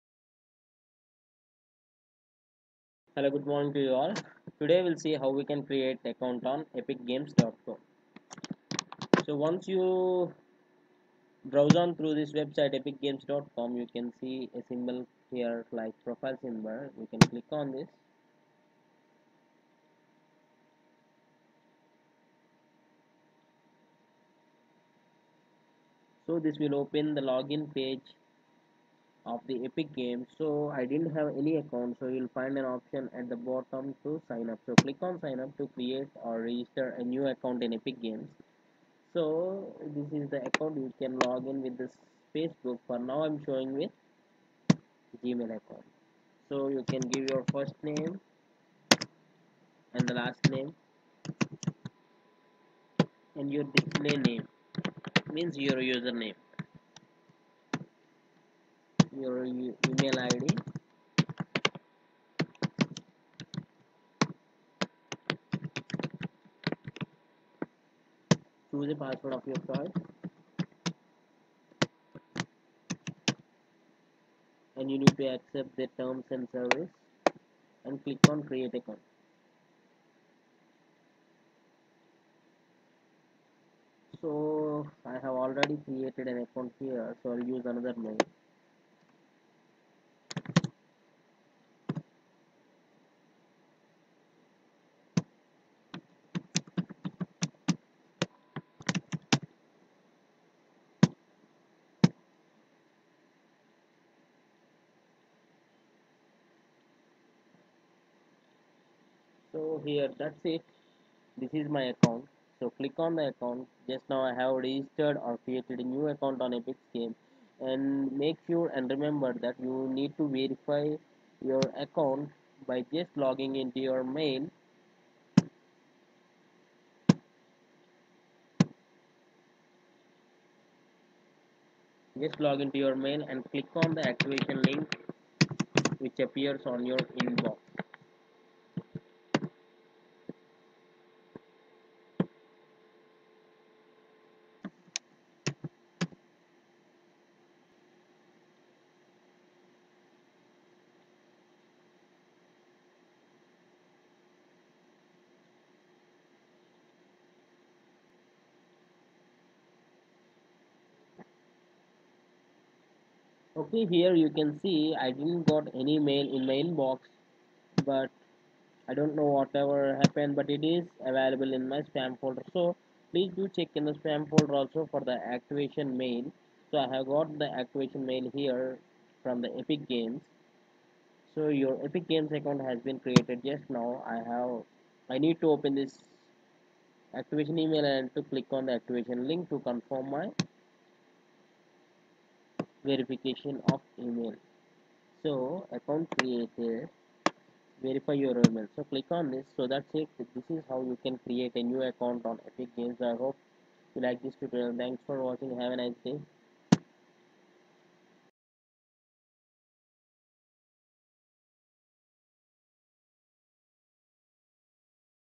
How to Create a Account on Epic Games 2018
Create a new account on Epic Games and creating or registering a account on epic games is very easy just follow the step wise video tutorial that shows how to create a new account on epic games 2018.